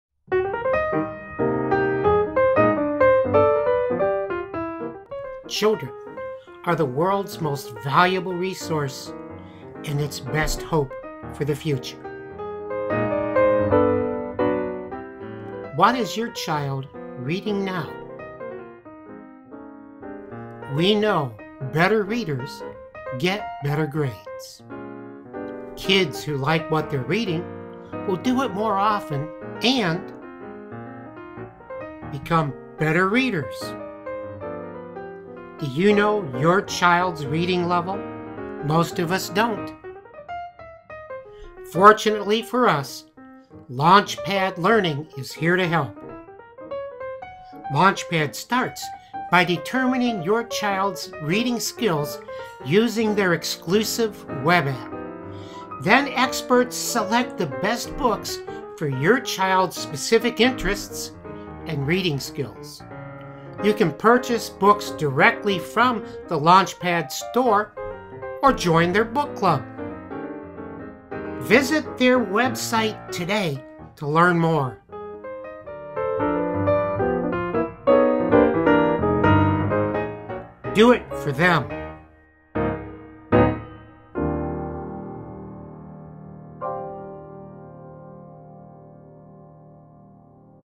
Our best hope for the future
Dr. Mark Winegar introduces LaunchPad Learning and their innovative strategy to improve reading skills.